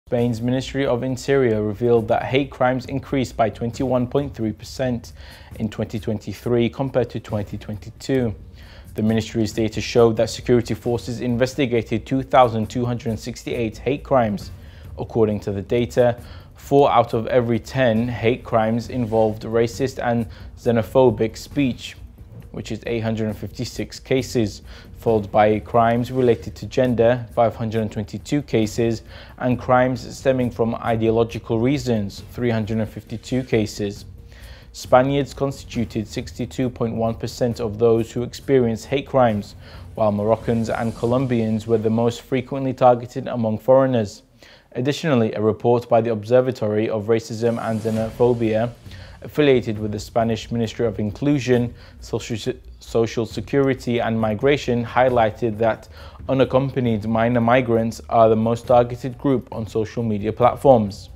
Spain: Hate crimes increase by 21.3% in 2023 compared to 2022
Spain's Ministry of the Interior revealed that hate crimes increased by 21.3% in 2023 compared to 2022. The Ministry's data showed that security forces investigated 2,268 hate crimes.
According to the data, 4 out of every 10 hate crimes involved racist and xenophobic speech (856 cases), followed by crimes related to gender (522 cases) and crimes stemming from ideological reasons (352 cases).
Spaniards constituted 62.1% of those who experienced hate crimes, while Moroccans and Colombians were the most frequently targeted among foreigners.
Additionally, a report by the Observatory of Racism and Xenophobia, affiliated with the Spanish Ministry of Inclusion, Social Security, and Migration, highlighted that unaccompanied minor migrants are the most targeted group on social media platforms.